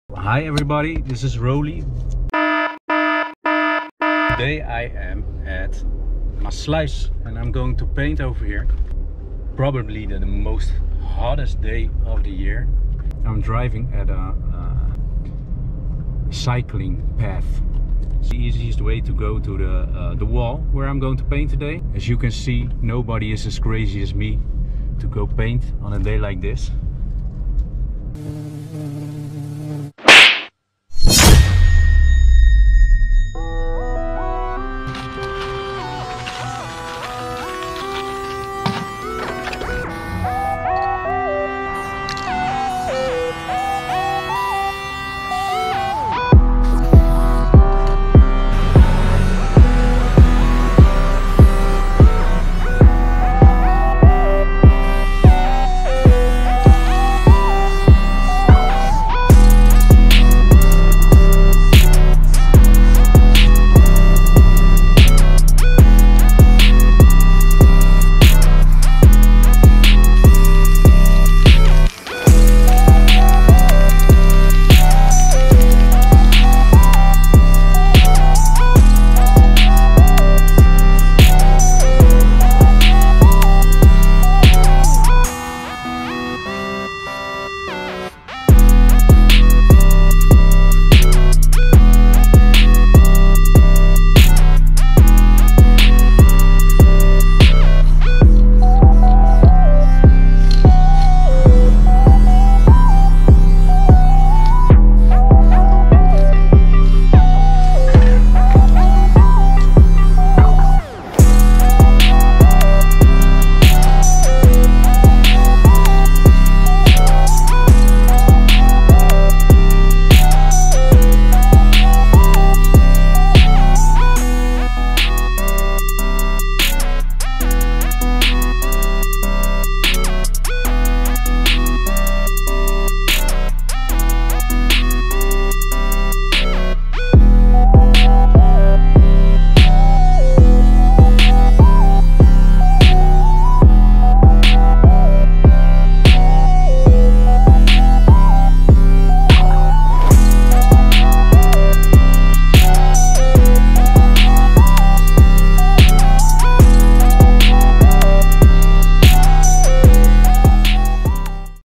Roley TV | painting a woman with a handgranate in Maassluis
Had a lot of fun creating this dangerous woman in Maassluis (The Netherlands).